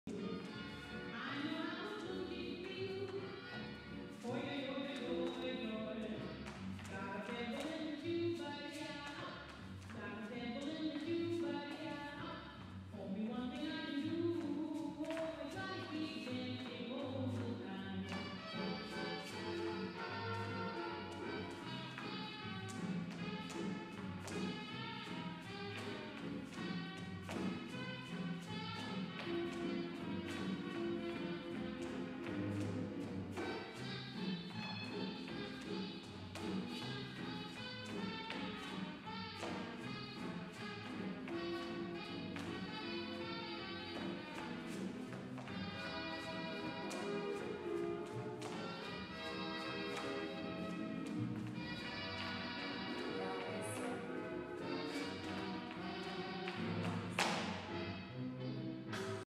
Lindy Hop Bases • Tandem Charleston (Shadow Charleston)
Estes passos de “Tandem Charleston” são aqueles em que o/a follower está à frente do/da leader na base de Charleston, como uma bicicleta “tandem” em que andam duas pessoas numa bicicleta, a fazer tudo em sintonia. Aqui fazem o passo com os mesmos pés no mesmo ritmo, a pessoa de trás como uma “sombra”, e por isso é que também se chama “Shadow Charleston”. Esta forma dá para ritmos diferentes, voltas e risos!
Professores Bruno Henriques & Carla Frade